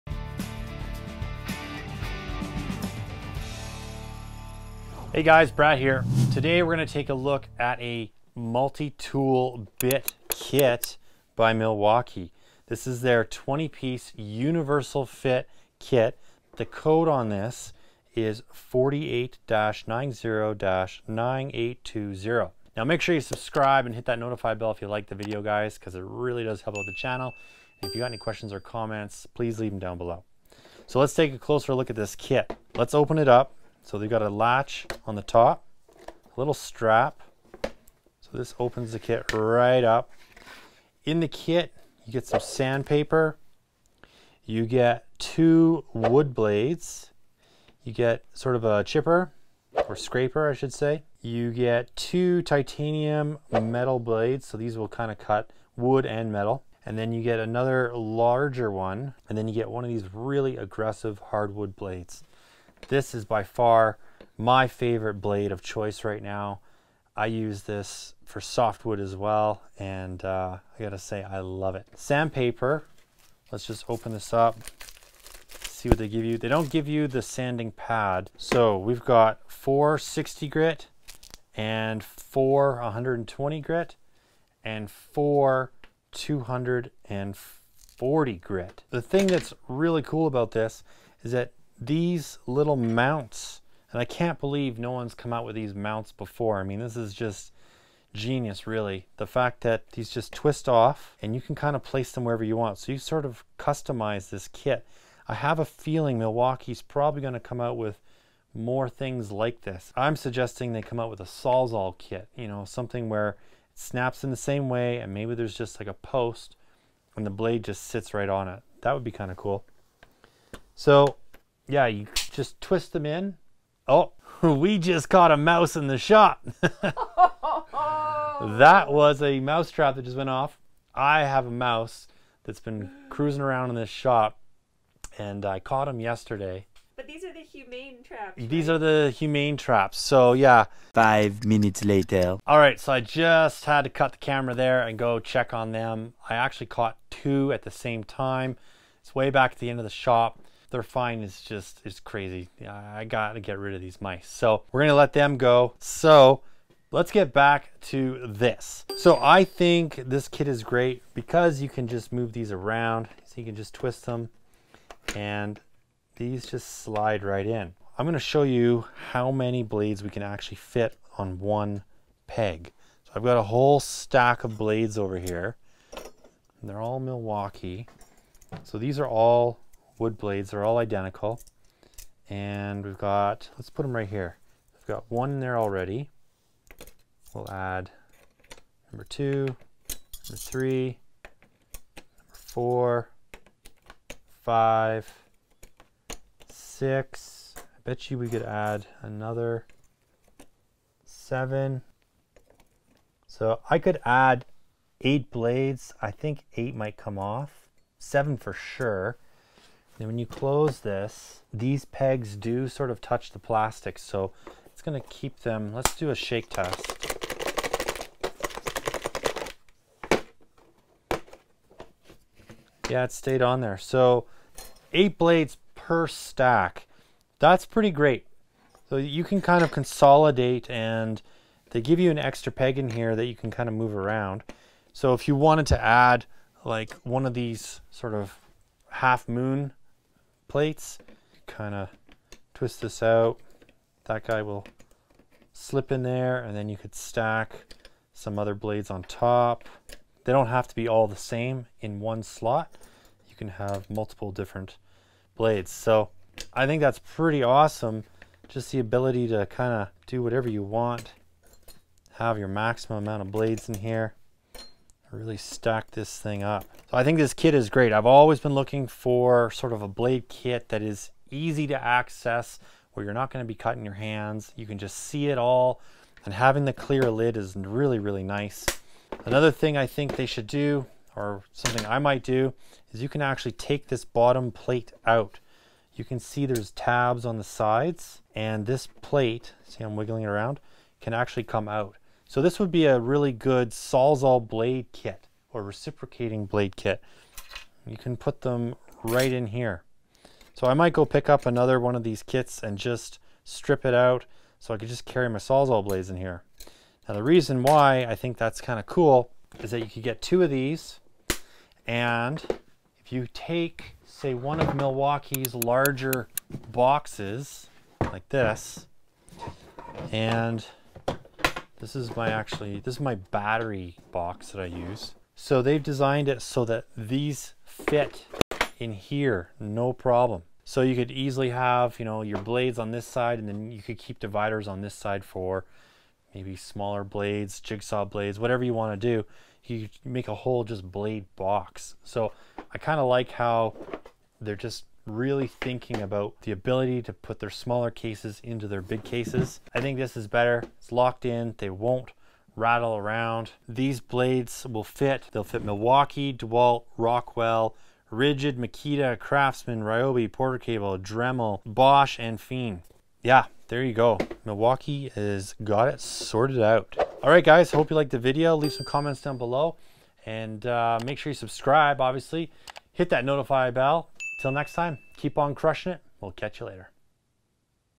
When the mousetrap goes off during a tool review!! Milwaukee Universal Fit Blade Set
Gotta tool question or home reno head-scratcher? Call Brad... with video! Brad is interrupted during his detailed review of Milwaukee Universal Fit Blade Set REVIEW!

Check it out Blade Sets on Amazon